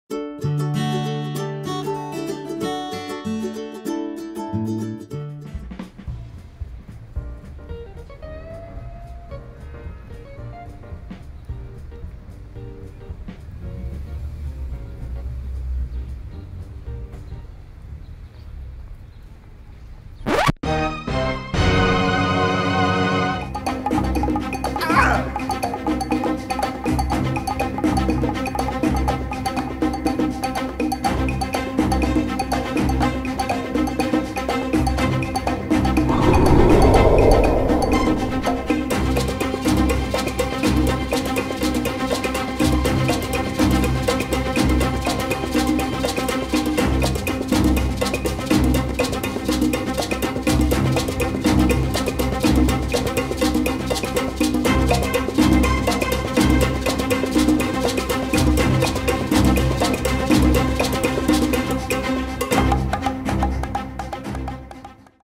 Der Bär mit Speer (Workshop Video)
Dieses Video wurde von Kindern im Rahmen eines Brickfilmworkshops am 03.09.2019 erstellt. Die Aufgabe war, den Clip mit der Legofigur in der Stadt fertigzudrehen.

Sounds: Freesound.org